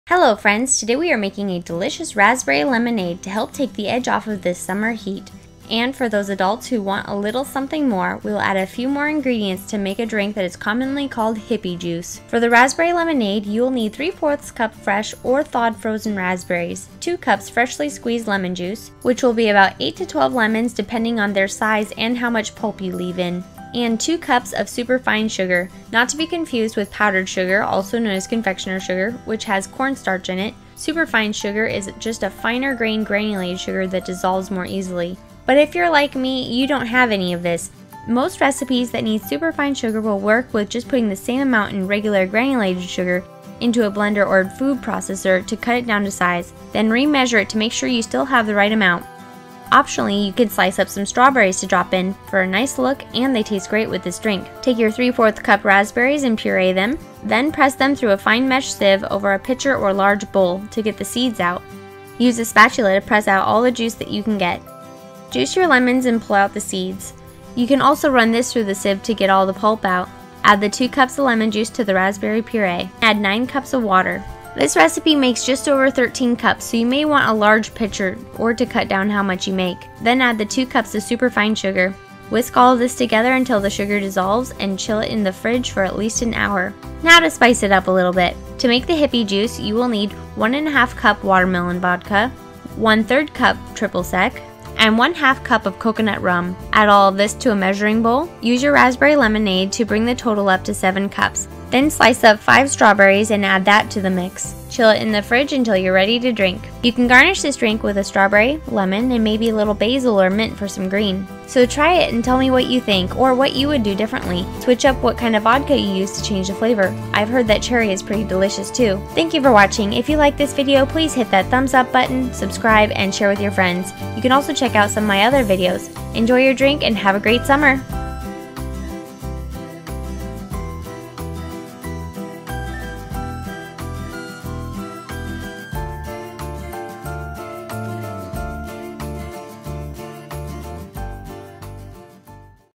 How to Make Raspberry Lemonade & "Hippie Juice" | Jenny Lynne
Make this refreshing summer drink that is easy to mix into the delicious "Hippie Juice", but without the pre-made mixes.